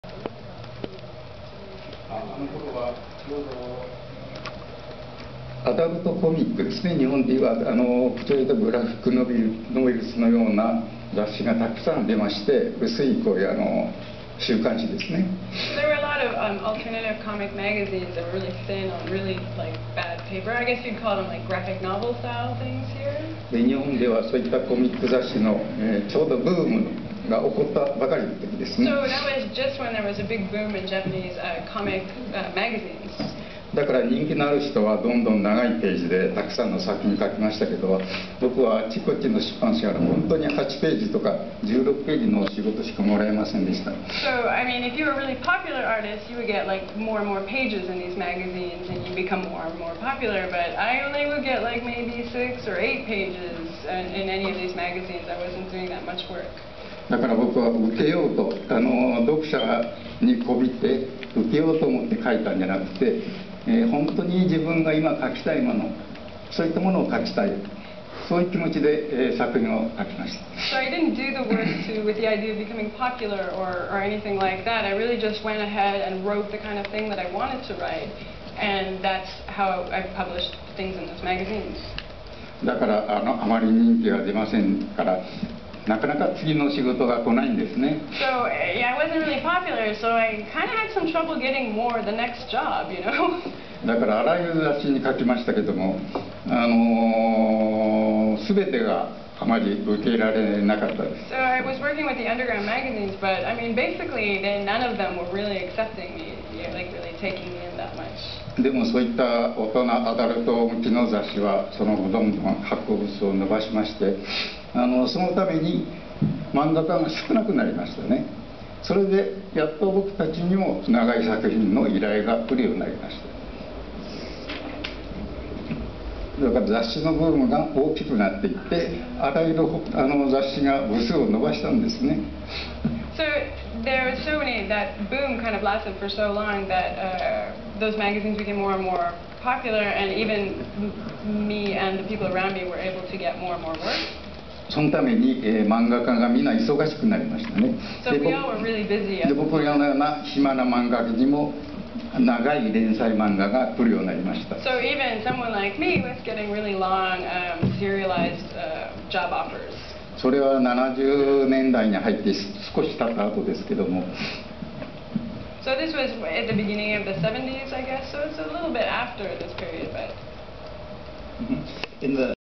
Yoshihiro Tatsumi interviewed by Adrian Tomine 1
Yoshihiro Tatsumi was interviewed on-stage by Adrian Tomine as part of the Toronto Comics Art Festival on May 8, 2009.

They spoke for 45 minutes about Tezuka, the early days of gekiga, and Tatsumi's thoughts on his massive autobiography A Drifting Life.